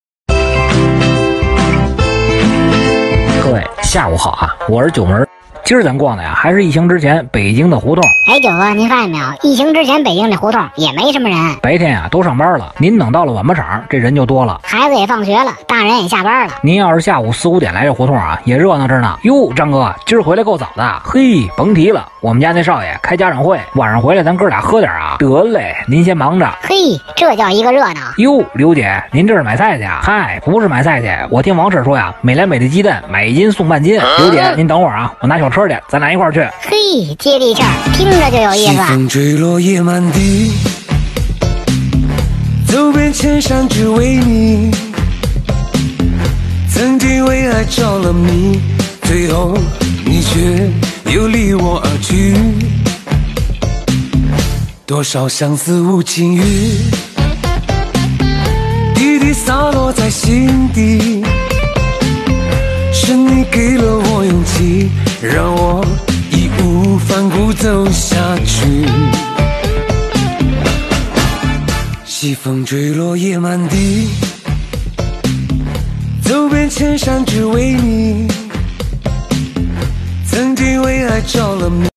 疫情之前空荡荡的老北京胡同，一个人也没有，您知道为什么吗 | 九门儿
用一个北京人的视角和记忆～带您发现不一样的北京城！